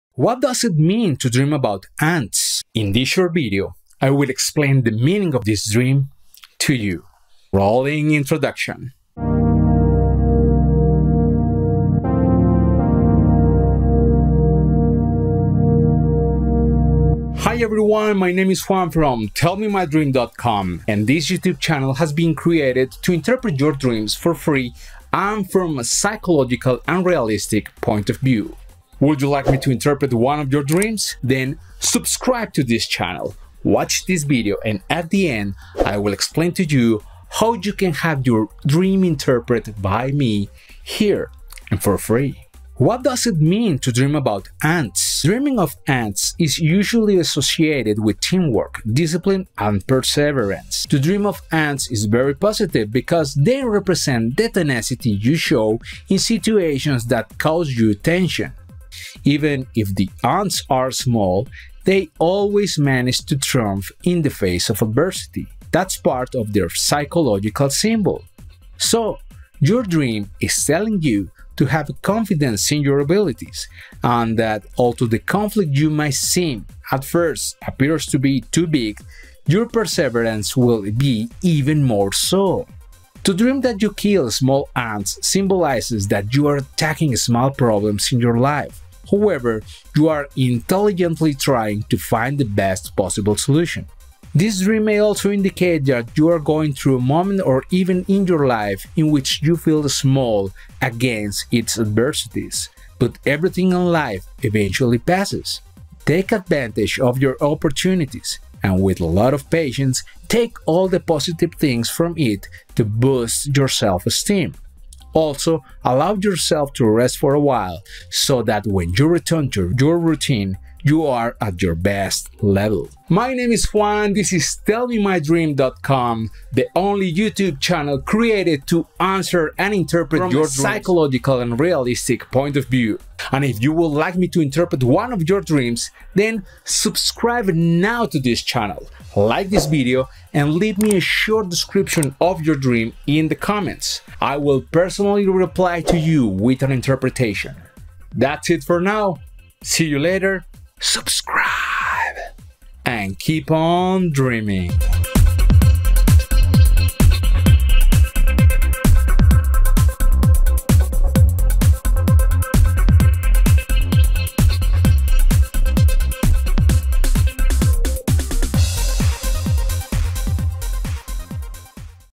ANTS Dream Meaning and Dream Interpretation
What does it mean to dream of ANTS?
What is the spiritual meaning of this dream? In this Short Video, Juan David Arbelaez from TellMeMyDream.com Explains in details the Psychological meaning and dream interpretation of this dream

Check us at:
to have your dream interpreted for free